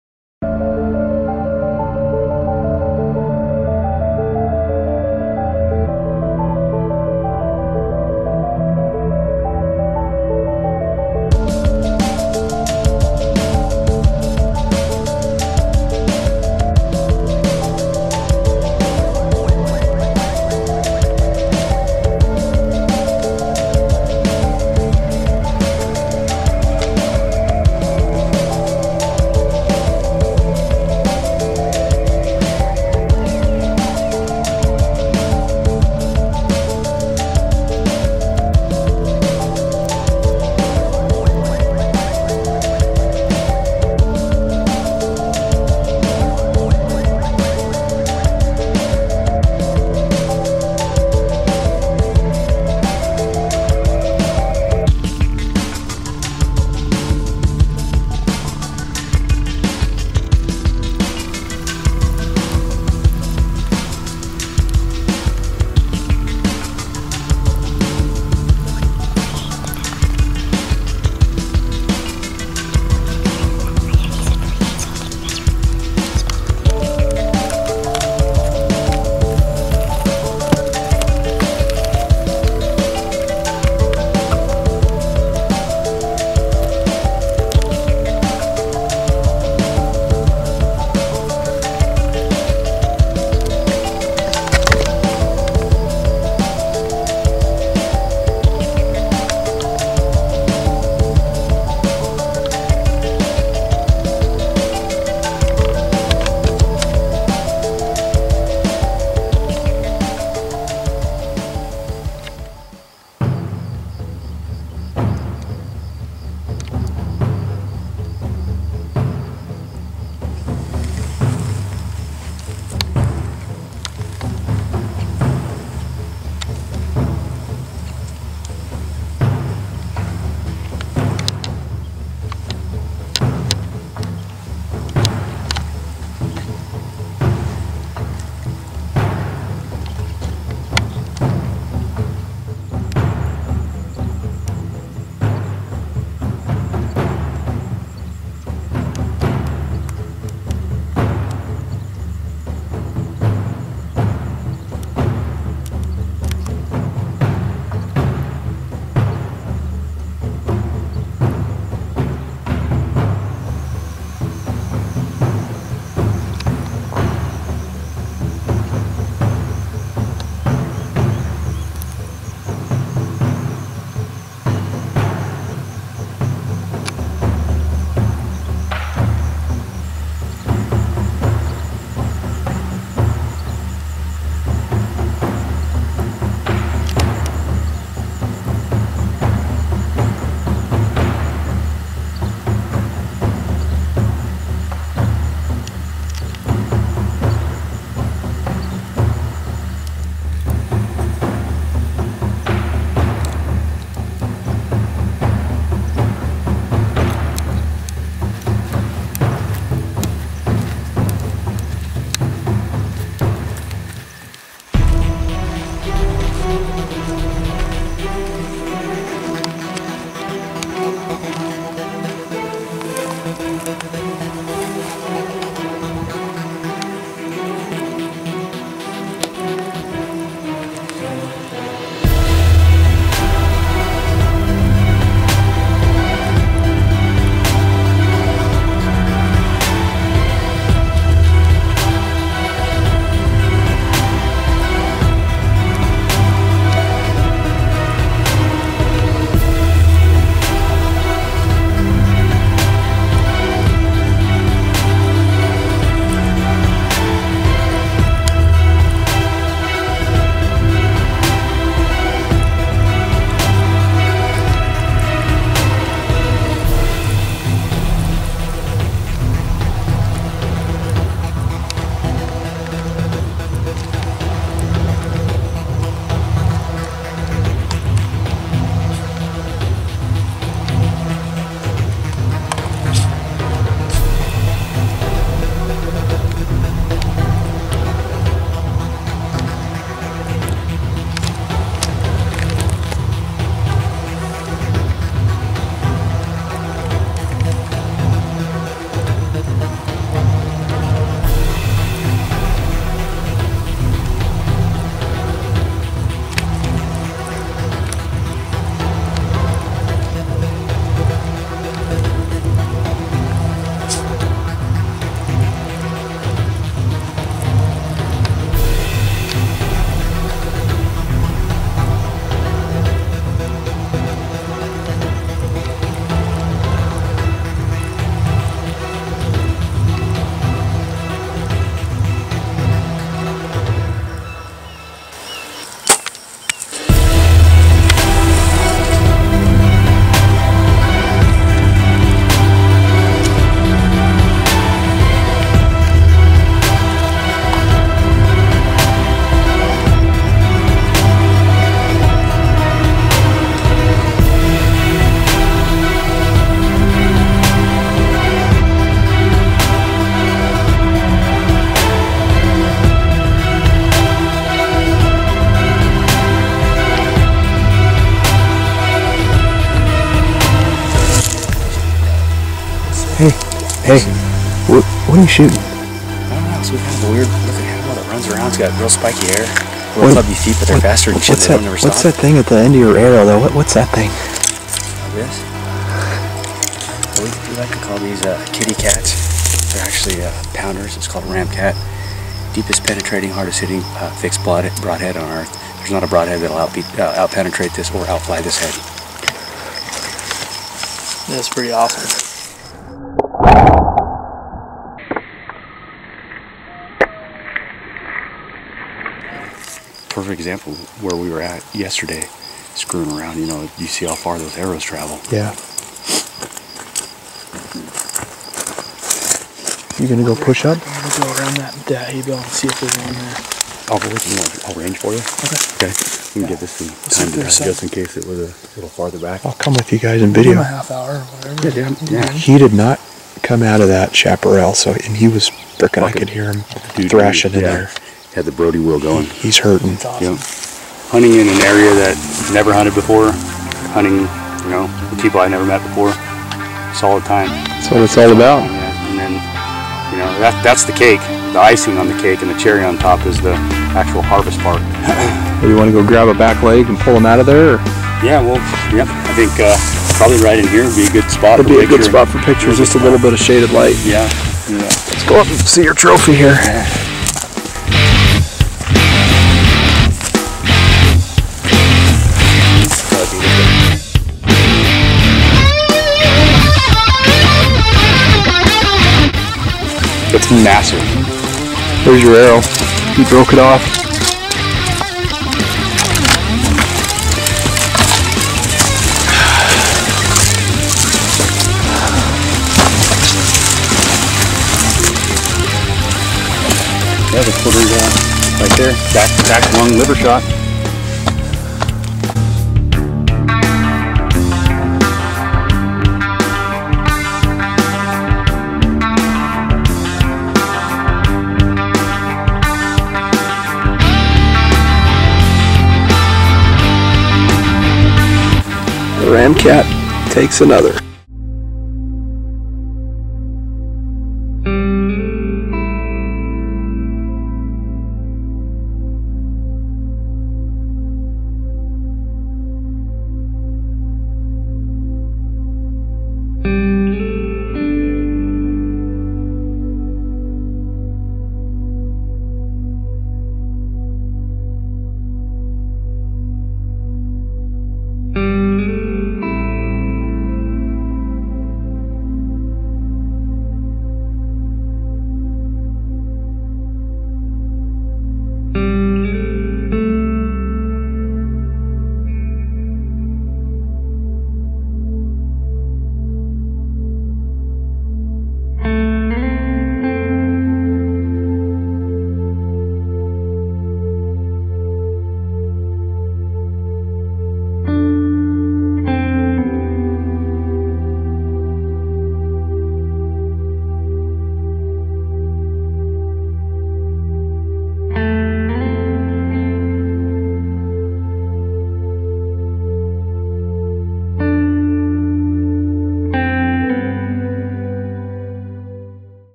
Redemption
Follow Micah Brown as he chases wild pigs on the Central Coast of California and gets redemption!